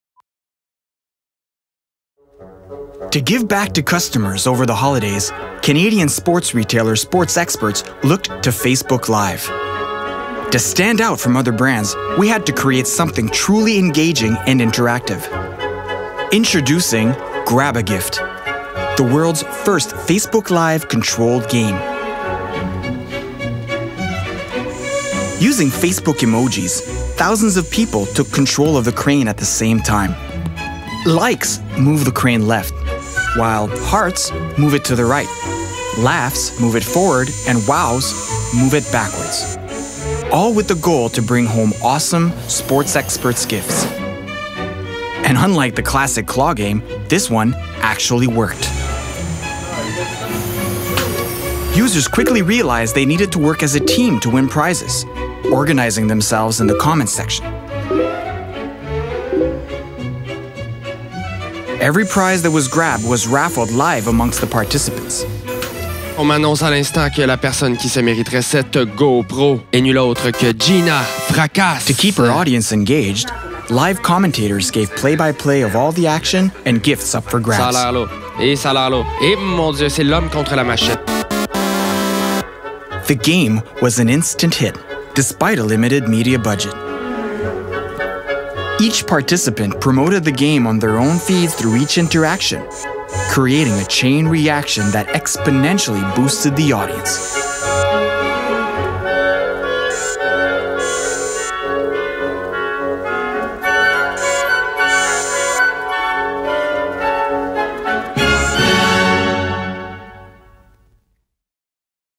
Sports Experts La grue à cadeaux
Design and fabrication of an interactive crane which moves according to Facebook reactions for Sports Experts, his agency Rethink and 1one production

Conception et fabrication d’une grue interactive qui se déplace selon les réactions Facebook des utilisateurs pour Sports Experts et ses agences Rethink et 1one production.

Robocut Studio
2017
Category
Science & Technology